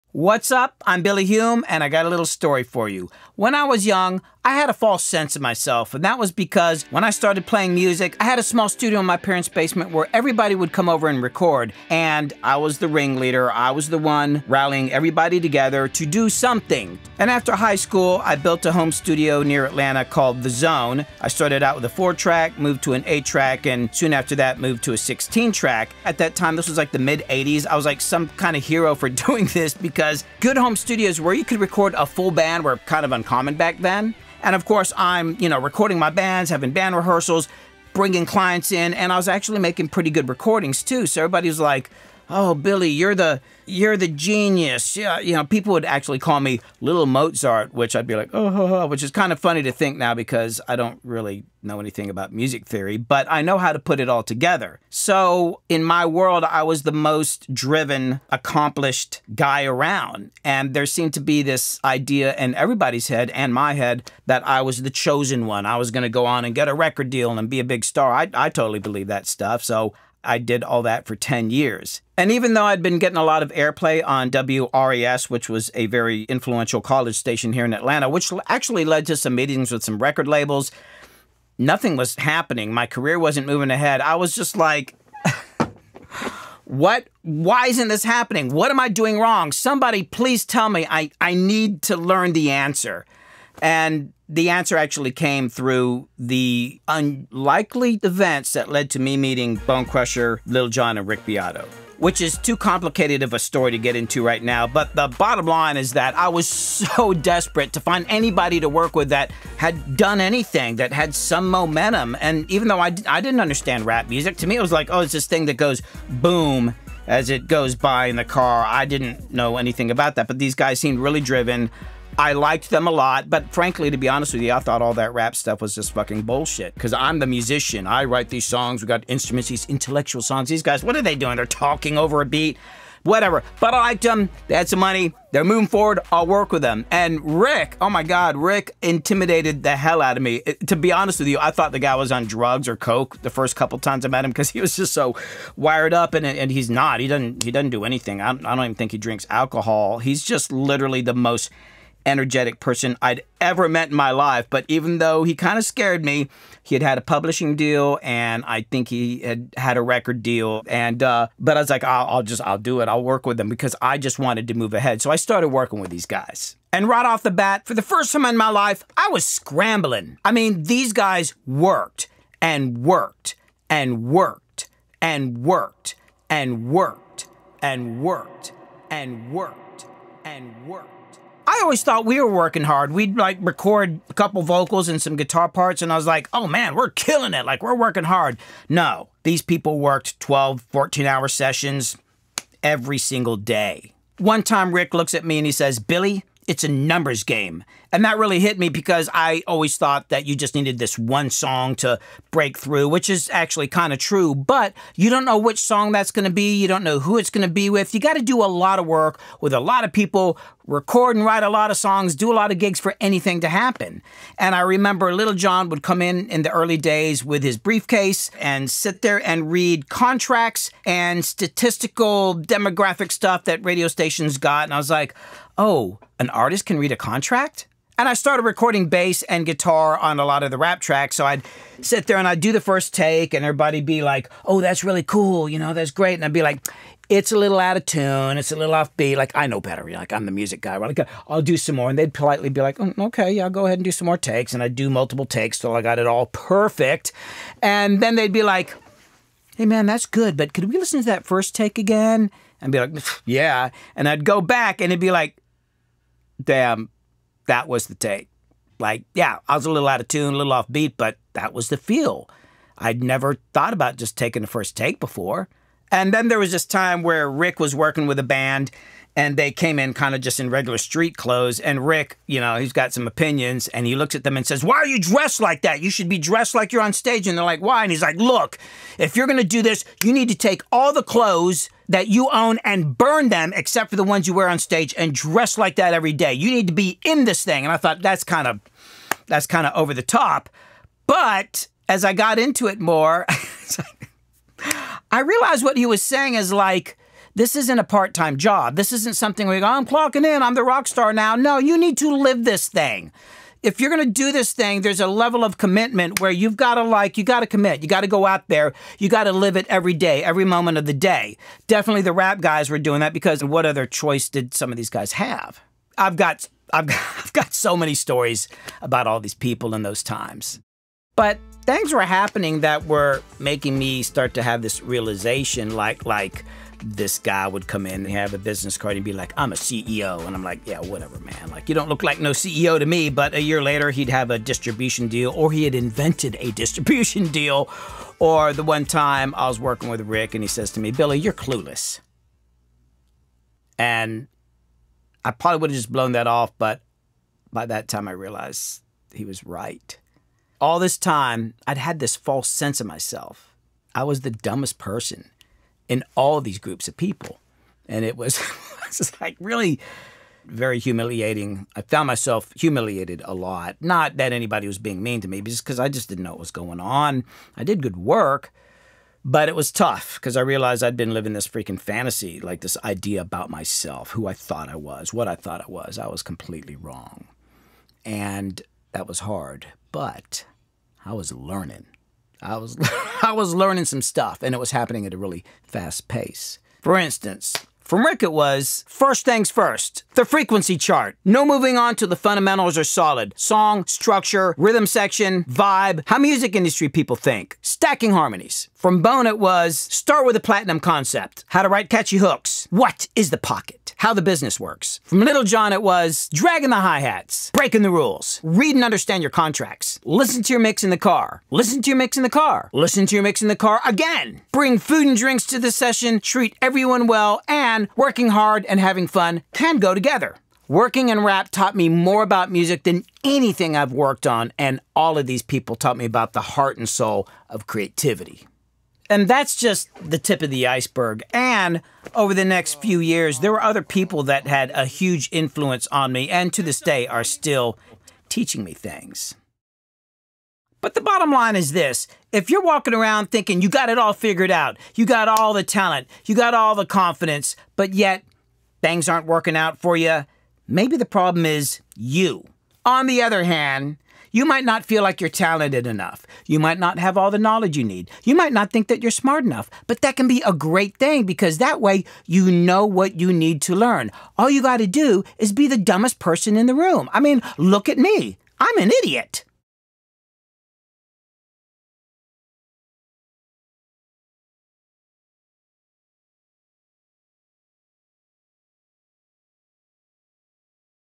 The Hardest Thing To Learn Was The Best Thing To Learn
The Hardest Thing To Learn Was The Best Thing To Learn .  That's what  learned from working with superstars like Lil Jon, Rick Beato and Bone Crusher and  was the best thing for me to learn as I was becoming a music producer.

A huge thanks to my supporters:
Ray Taylor
Kevin P.
Crystal Whedbee
Chris Vermillion